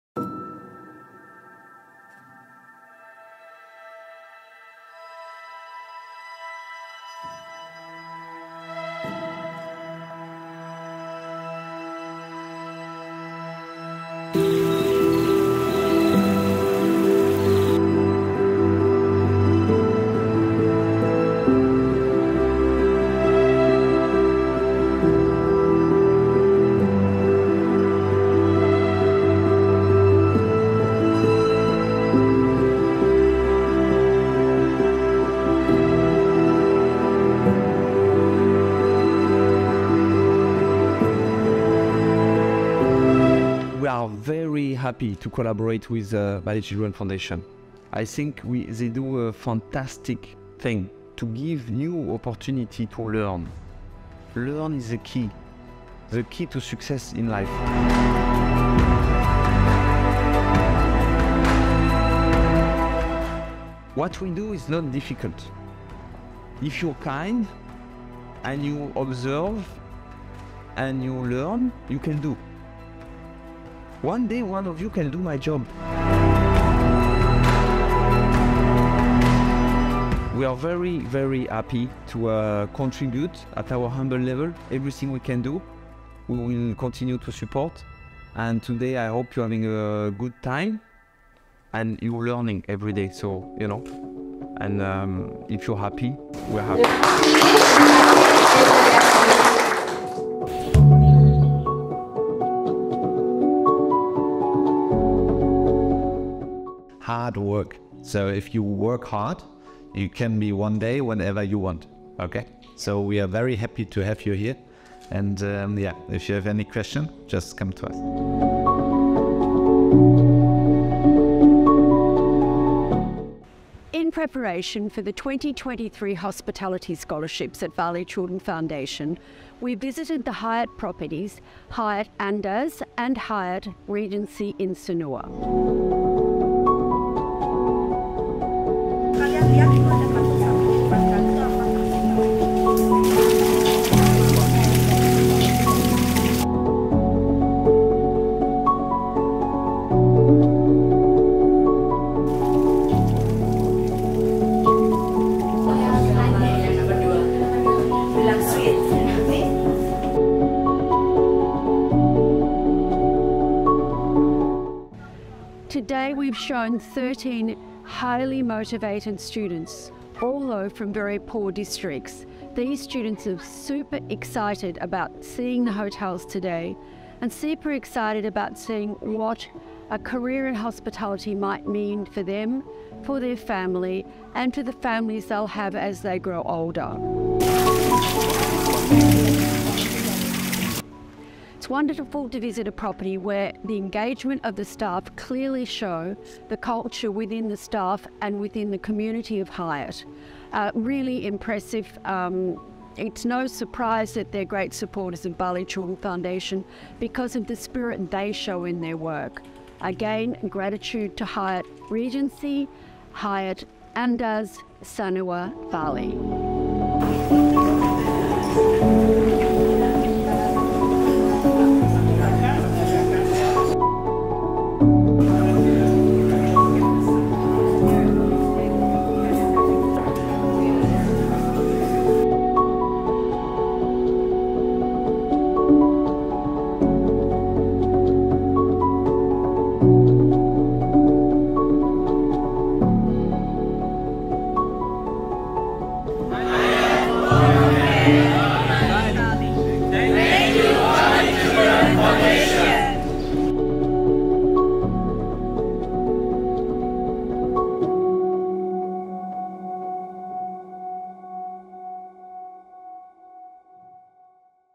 Empowering Dreams: Hyatt Hotels 2023 Hospitality Scholarships
In collaboration with the esteemed Hyatt Hotels, we had the privilege of preparing 13 talented students from underprivileged backgrounds for the 2023 Hospitality scholarships at Hyatt Andaz and Hyatt Regency in Sanur. The student's enthusiasm for a career in Hospitality was contagious, as they recognized its life-changing potential for themselves and their families. The unwavering dedication of the Hyatt staff, reflecting a vibrant culture within the company and the surrounding community, further exemplifies their strong support for our organization. We extend our sincere gratitude to Hyatt Hotels for their generosity in opening their establishments to our scholars, making a meaningful impact on the lives of our students and their families. Together, we are paving the way for a brighter future. Don't miss this inspiring journey!